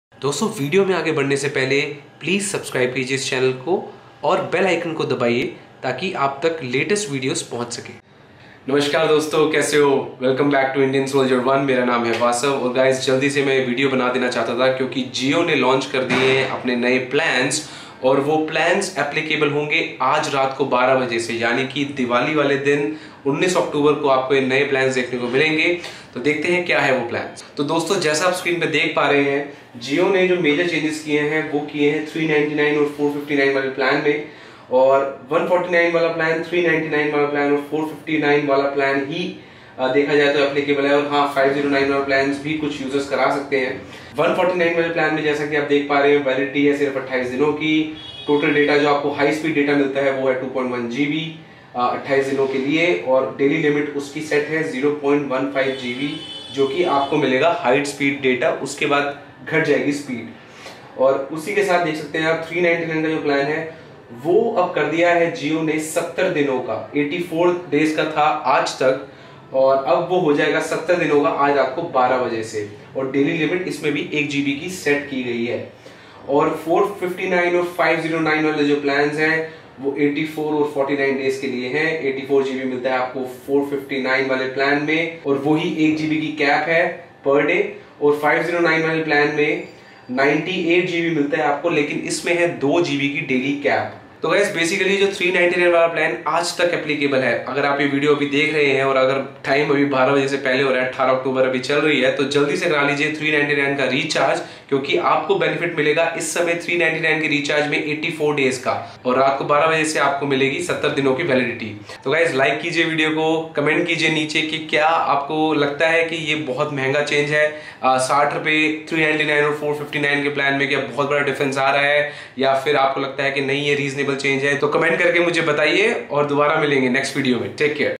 हो गए Reliance JIO के प्लान्स मेहेंगे | Jio Ka DIWALI Jhatka 2017, Dhan Dhana dhan
हो गए Reliance JIO के प्लान्स मेहेंगे | Jio Ka DIWALI Jhatka 2017

Like my Facebook page for all updates related to this channel